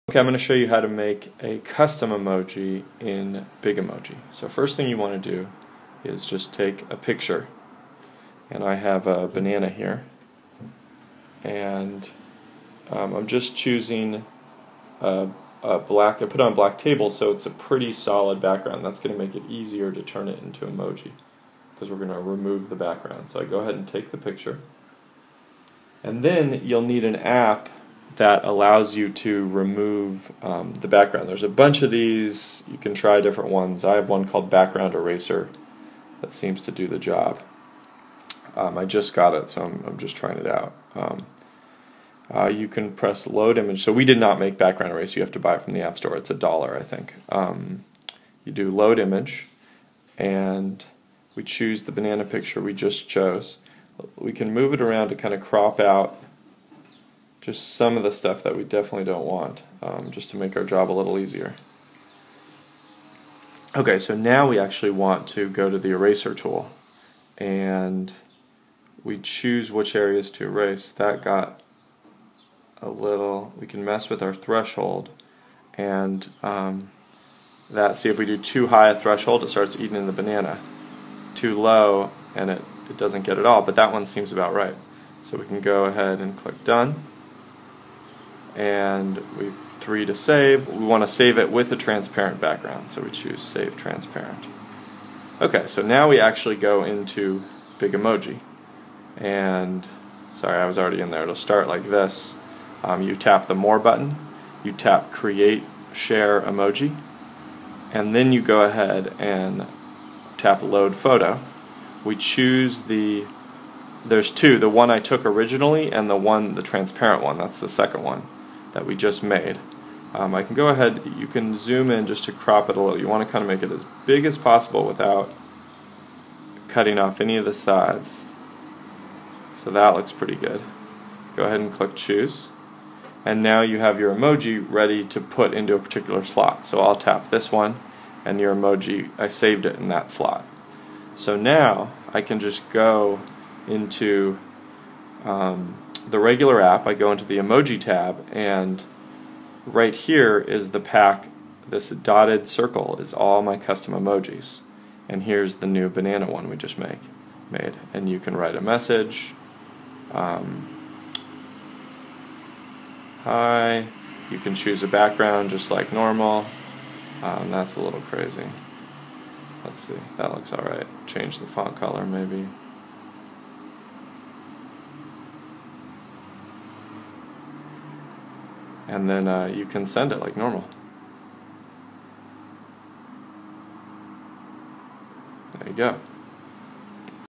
How to create a custom Big Emoji
If you want to make the highest quality custom emojis, you'll need an app that can erase the background of photos. This video walks you thru an example using an app called "Background Eraser".

Note: If you don't remove the background from your photo, Big Emoji will try to do it on its own. But for it to work, the background of the photo will have to be a very solid background.